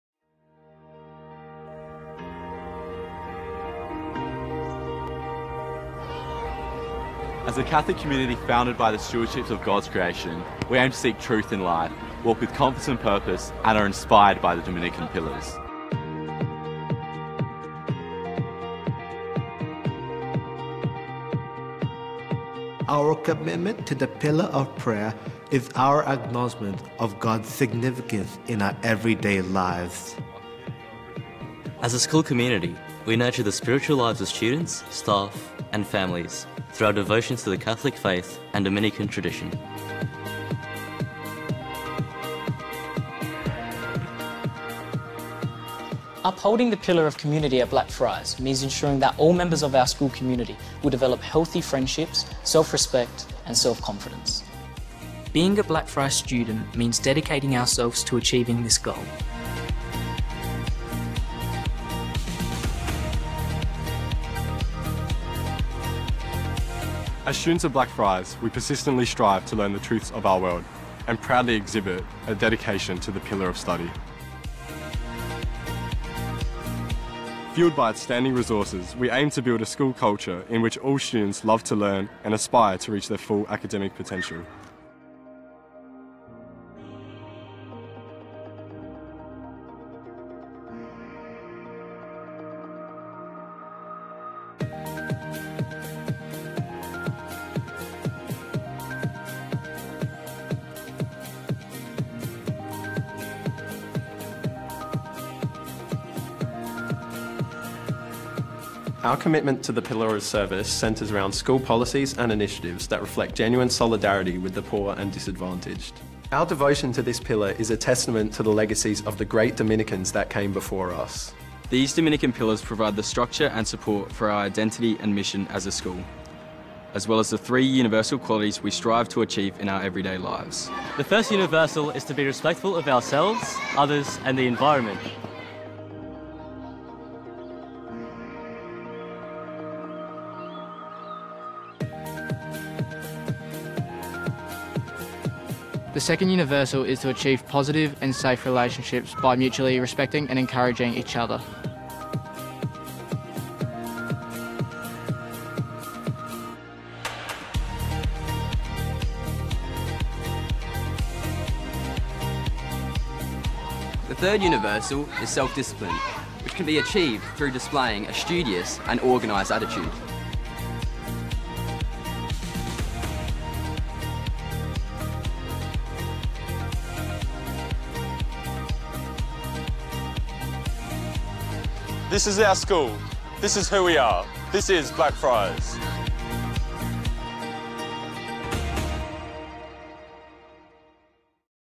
남호주 명문 사립학교 - 블랙프라이어스 스쿨(Blackfriars Priory School)
남호주의 남자 사립학교 블랙프라이어스 스쿨을 소개합니다.